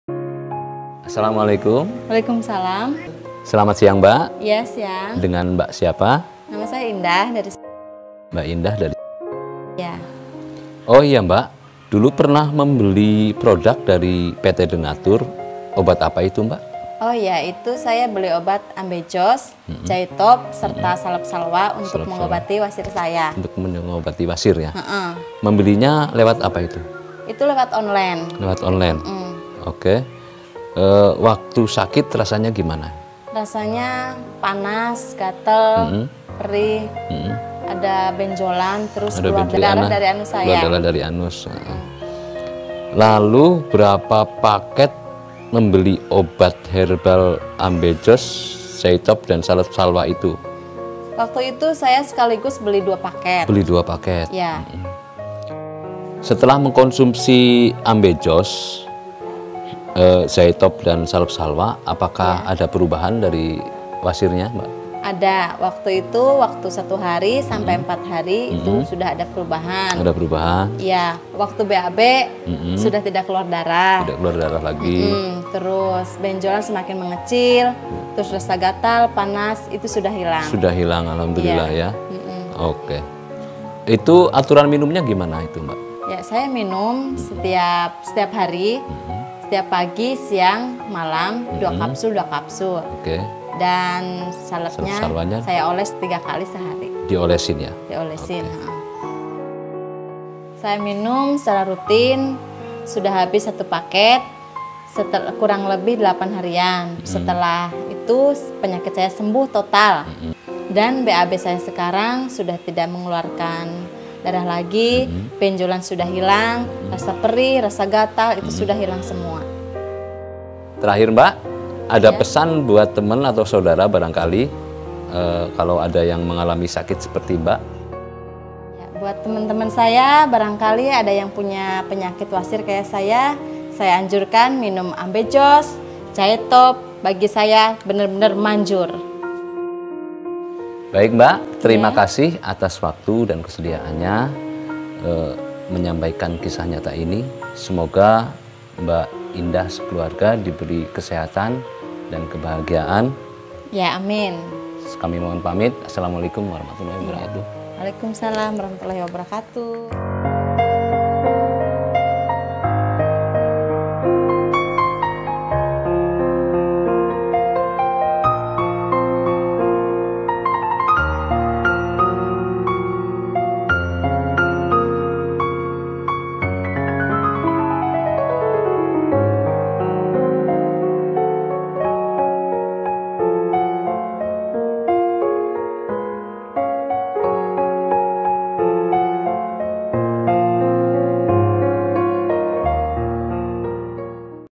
Testimoni wasir/ambeien yang sudah sembuh
WASIR​ merupakan keluhan yang umum terjadi. Penyakit dengan nama lain hemorrhoid ini ditandai dengan munculnya benjolan pada anus yang bisa disertai nyeri atau gatal pada anus, serta perdarahan saat BAB. Gejala dan tanda yang muncul bisa sedikit berbeda, tergantung pada jenis ambeien yang dialami.